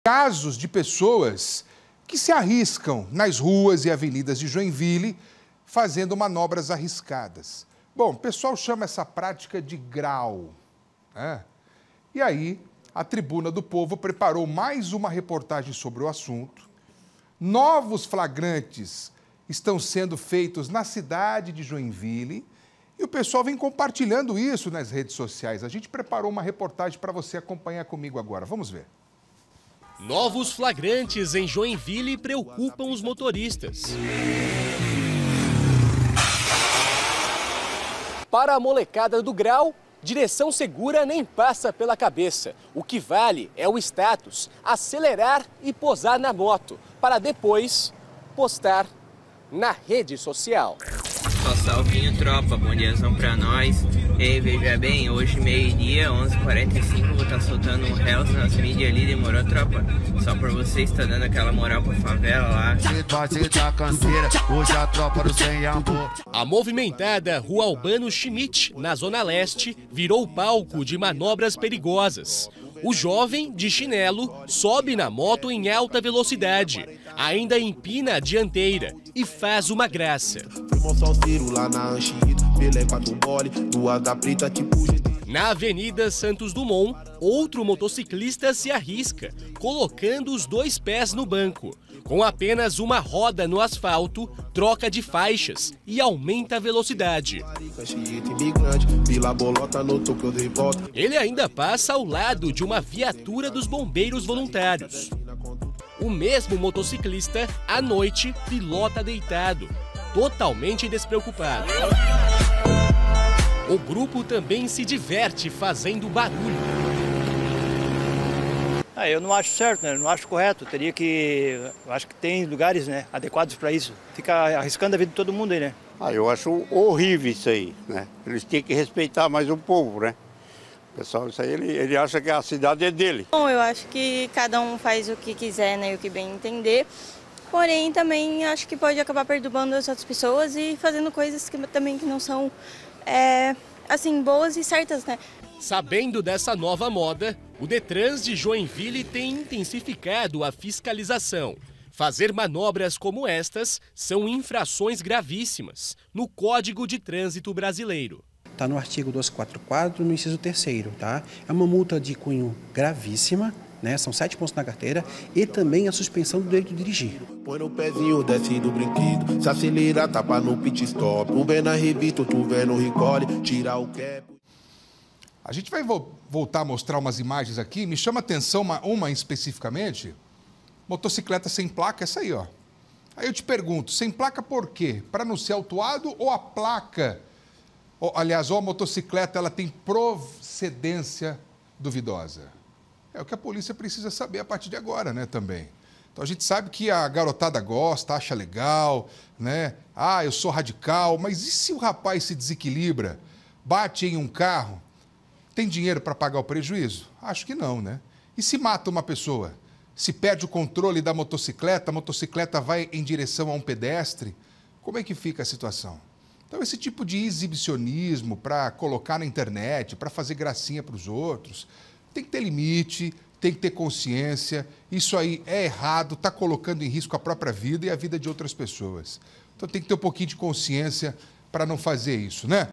Molecada do grau em Joinville: ação irresponsável coloca vidas em perigo
Na cidade de Joinville, Santa Catarina, uma prática perigosa tem preocupado os motoristas e colocado vidas em risco no trânsito. Conhecida como "molecada do grau", jovens realizam manobras arriscadas com suas motocicletas, colocando em perigo não apenas a própria segurança, mas também a de pedestres e outros motoristas. Nessa matéria de vídeo, mostraremos flagrantes recentes dessa prática irresponsável e discutiremos as medidas que estão sendo tomadas para coibir essas ações. A segurança no trânsito é responsabilidade de todos, e é preciso conscientizar os jovens sobre os riscos que suas atitudes podem causar.